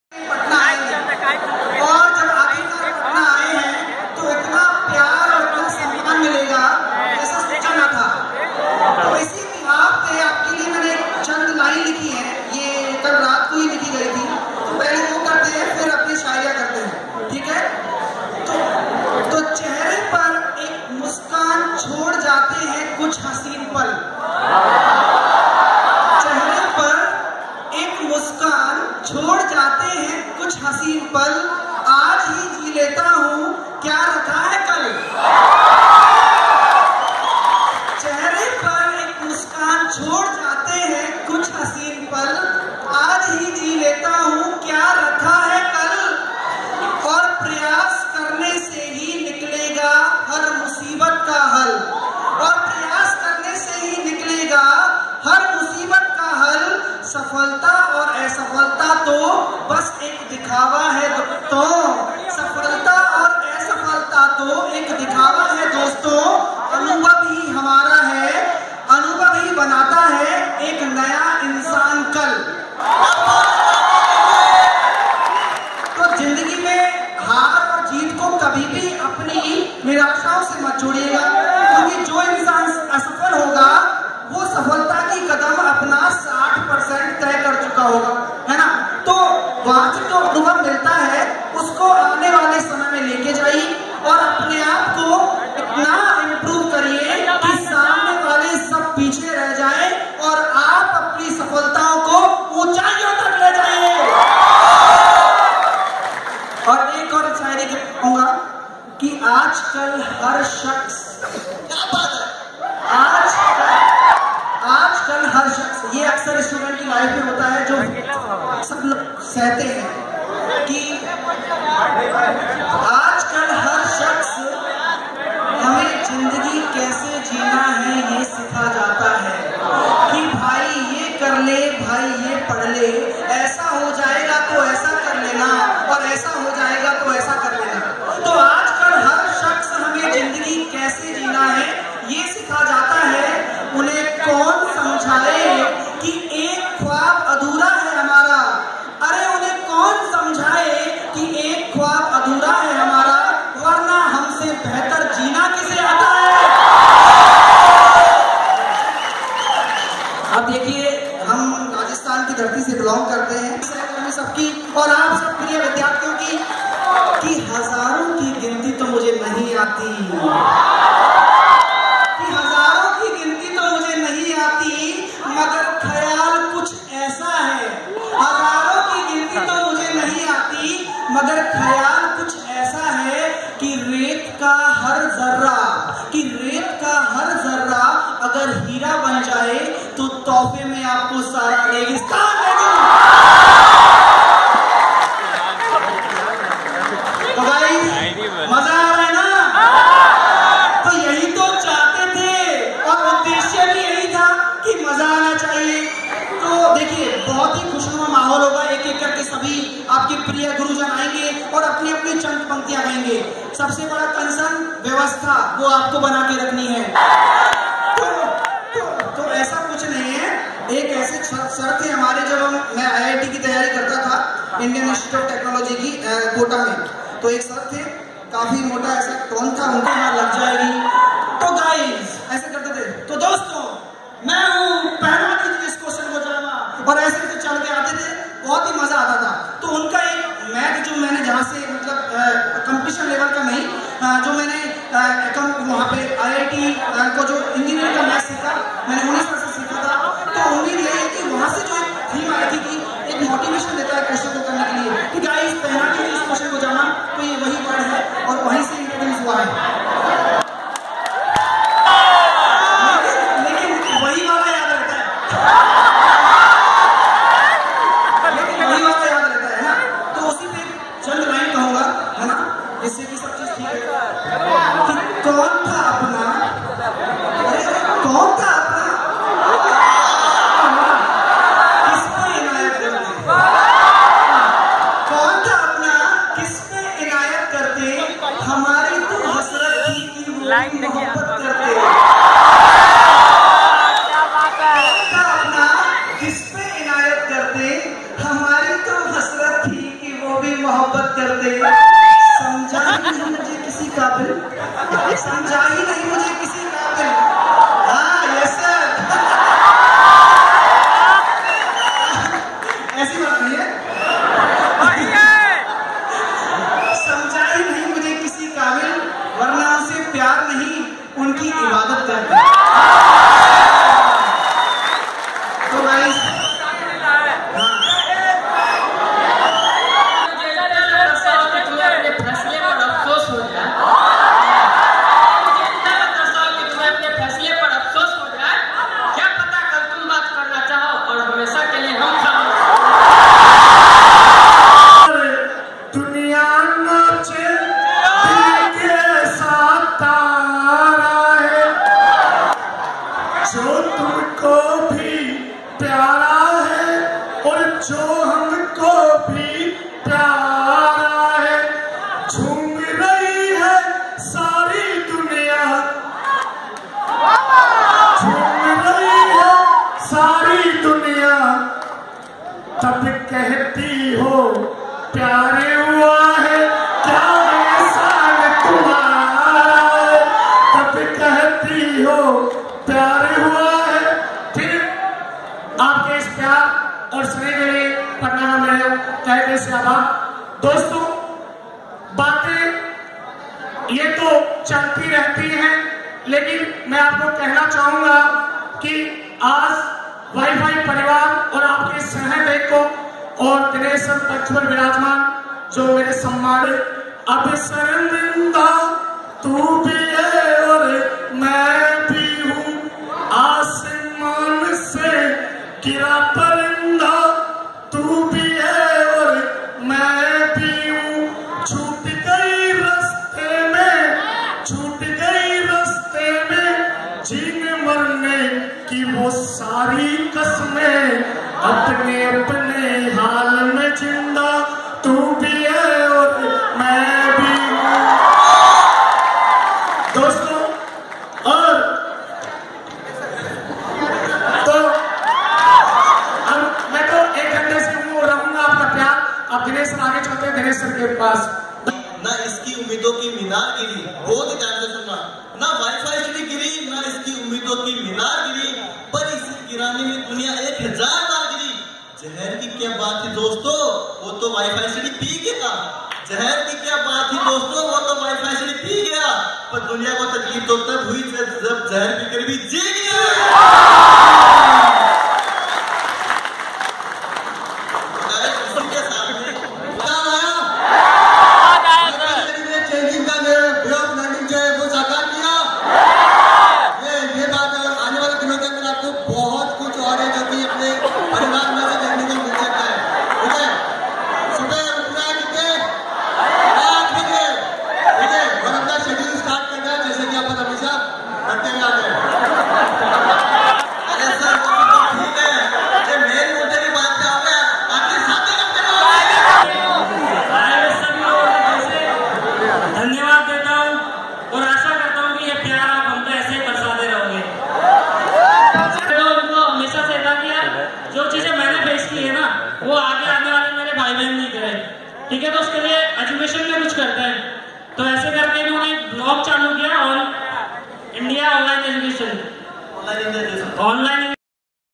wifistudy # patna# Sandeep sir Gk Sahil sir hitesh mishra# shipra mam
WIFI-STUDY 5M CELEBRATION PATNA

Sahil Sir Shayari

IN PATNA 1 DECEMBER 2K18 SATURDAY. ALL TEAM CAME IN PATNA .FULLY ENJOY ,THANKS ALOT WIFISTUDY TEAM.WHAT A WONDERFUL MOMENT SIR WHICH I SPEND WITH U ..